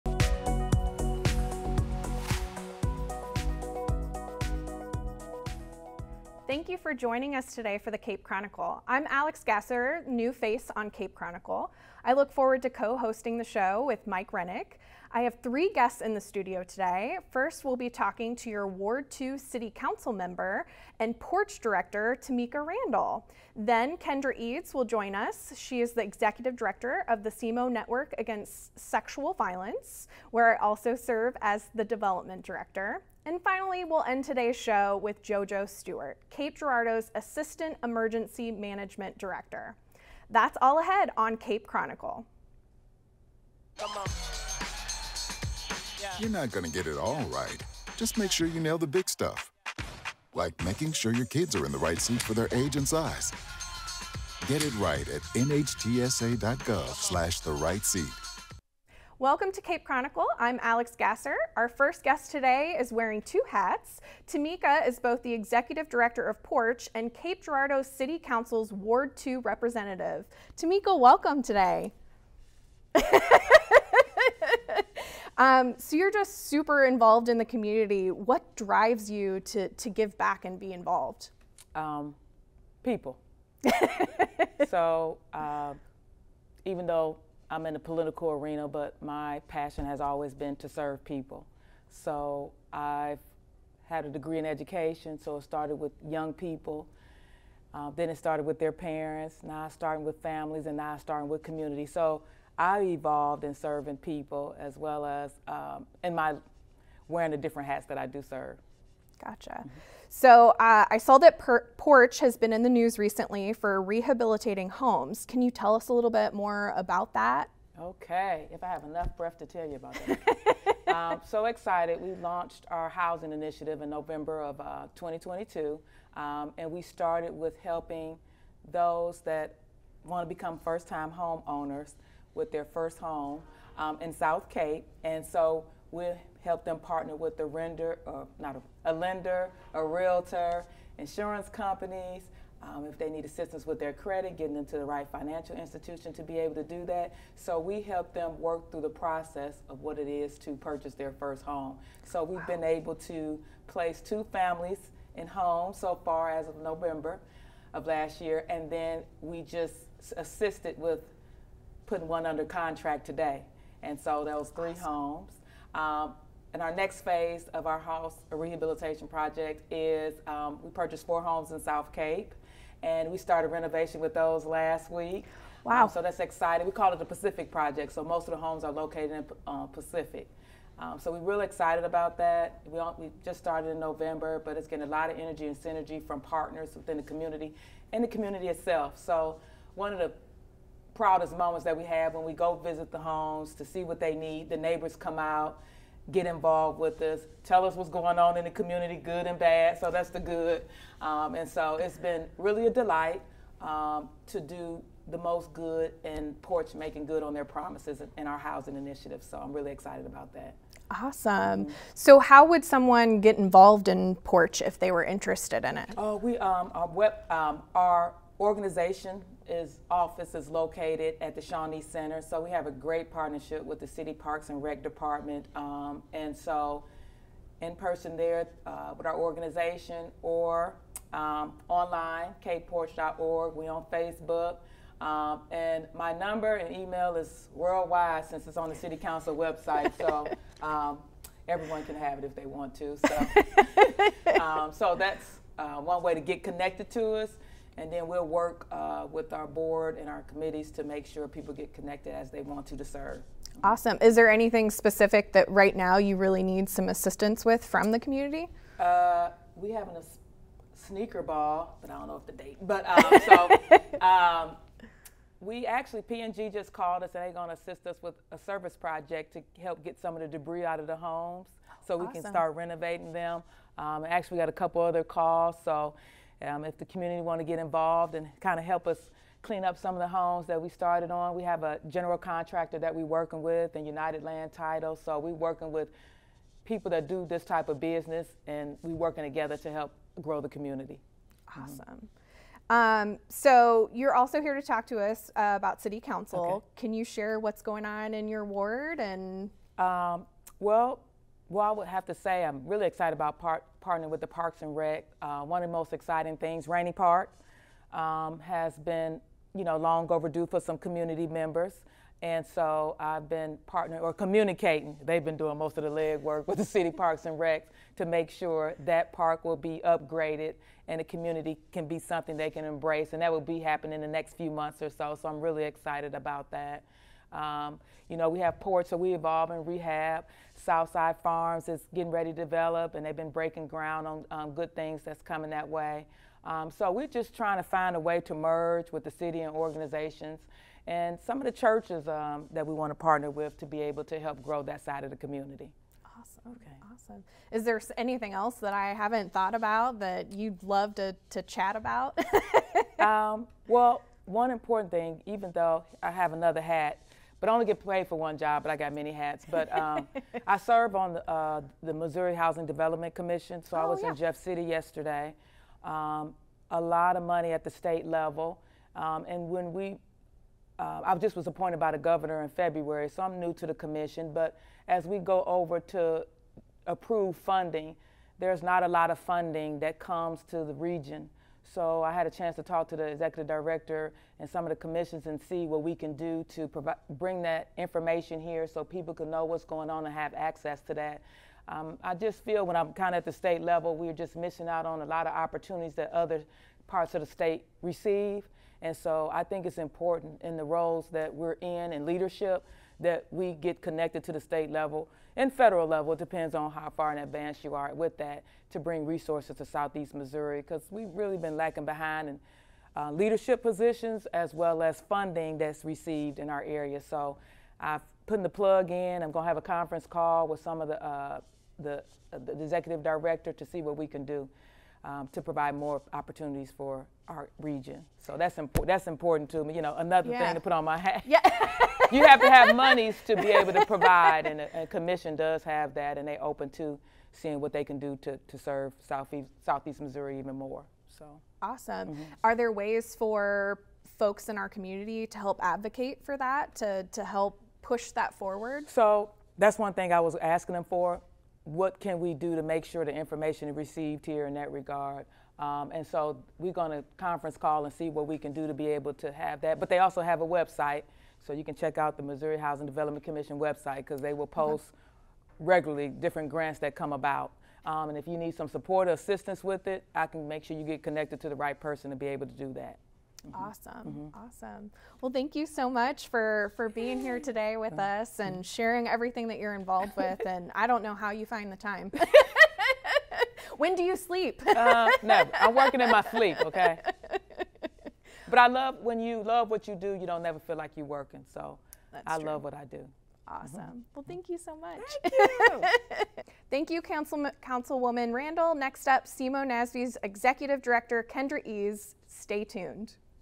Tameka Randle - City & PORCH Leaders (Cape Chronicle 9/23)
Tameka Randle discusses her many leadership roles with new Cape Chronicle Co-Host Alix Gasser.

Tameka Randle is currently serving as...
* Executive Director of People Organized to Revitalize Community Healing (PORCH)
* Ward 2 Cape Girardeau City Council Representative
* Governor Mike Parson appointee to the Missouri Housing Development Commission (MHDC)

More about our Councilwoman:

ELECTED
April 2022

EDUCATION
Master of Public Administration, Southeast Missouri State University
Bachelor of Science, Eastern Michigan University

COMMUNITY INVOLVEMENT:
•Missouri Community Service Commission, former Commissioner
•Missouri Chamber of Commerce and Industry, Leadership Missouri Alumni Board Member
•Department of Higher Education and Workforce Development, former Equity Advisory Committee Member
• Southeast Missouri State University, former Endowed Scholarship Committee Member
•Gibson Center, Board Director, President and Executive Committee
•Cape Girardeau Regional Airport, former Advisory Board Member
•Cape Girardeau Area Chamber of Commerce, Alumni Association